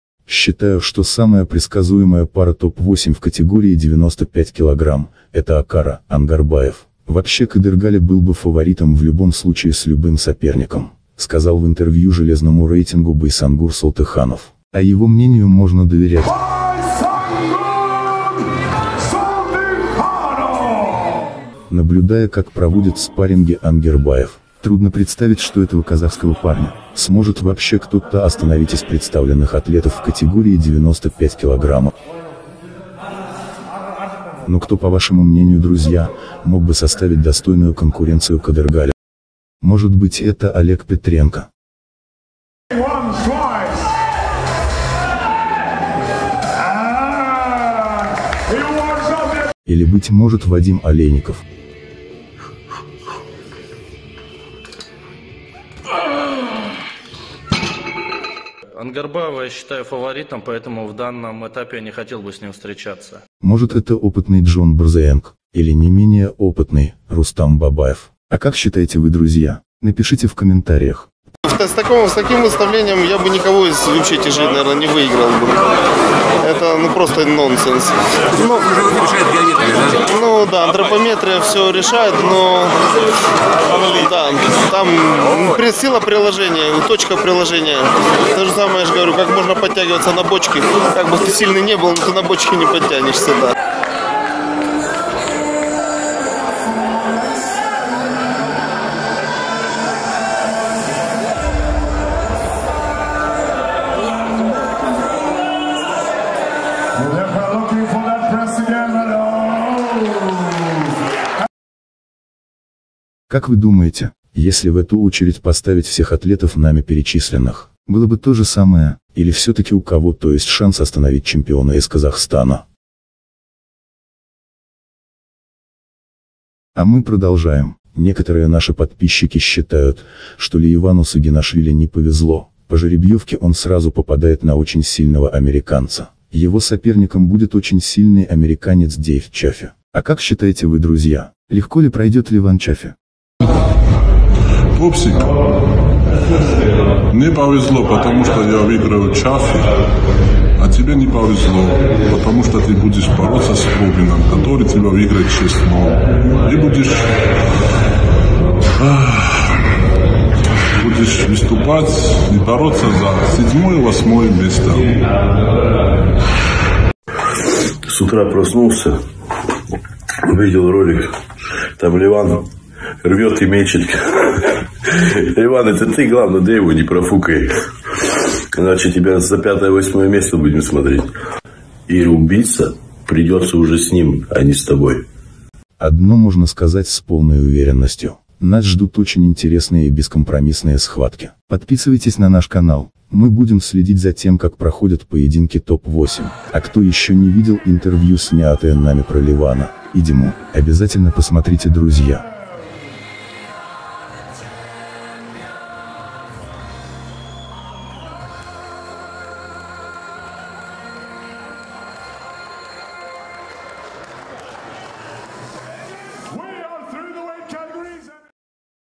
Кто остановит рукоборца из Казахстана
Кто остановит КЫДЫРГАЛИ ОНГАРБАЕВА? Выиграет ли Леван Сагинашвили у ДЕЙВА ЧАФФИ
Для безвозмездной поддержки инвалида спортсмена выезжающего на чемпионат Европы: Номер карты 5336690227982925 сбербанк.